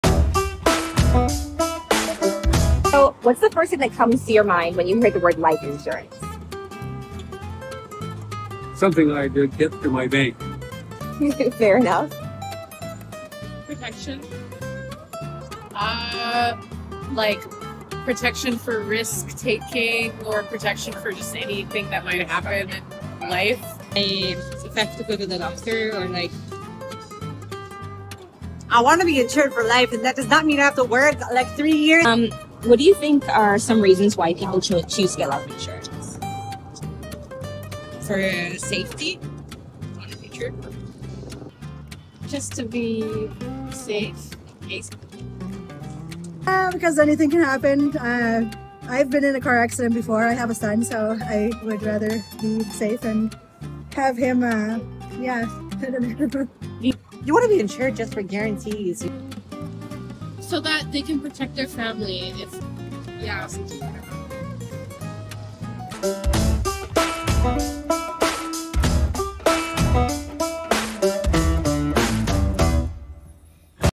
Speaking to Canadians about Life Insurance
Many people might not have life insurance yet, but they are spot-on about why people choose to get it.